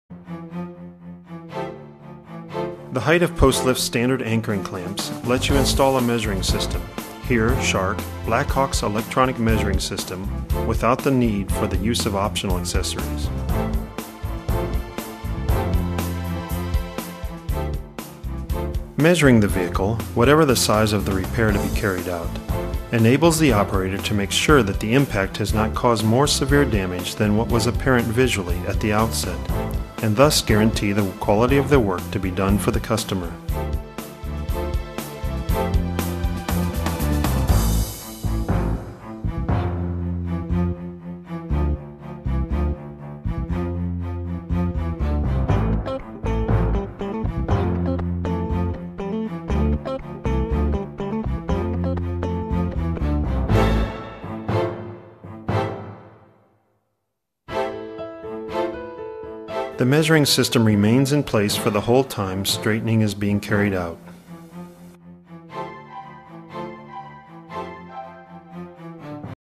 Blackhawk Shark - electronic measuring system
overview of how the Shark measuring system operates